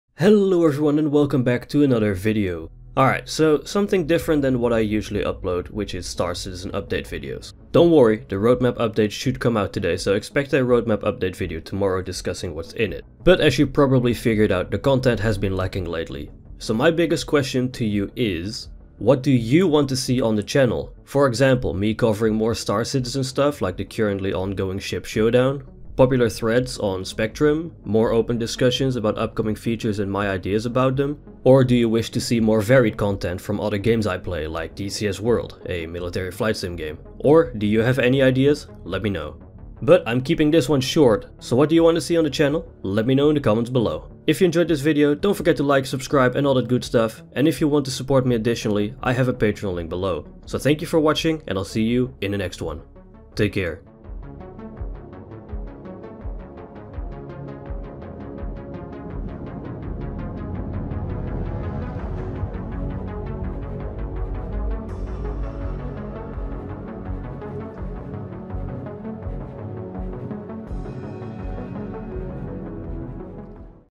What do YOU want to see on the channel ?!
what do you want to see more of on the channel?

More Star Citizen related content? More variety like DCS: World or other games?